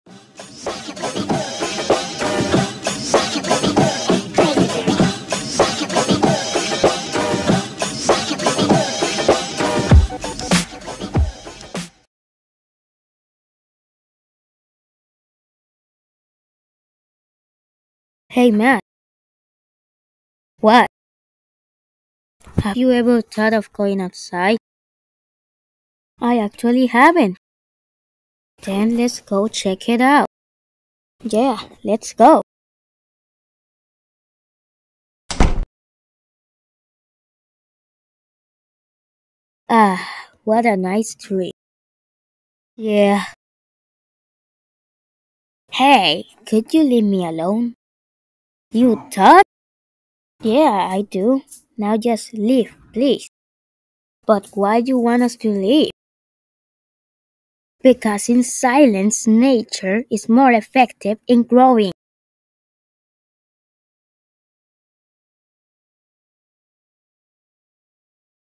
A Ballin Series Episode 3: Outside
Matt And Nattan Go Outside For The First Time! (Aka First Time Touching Grass)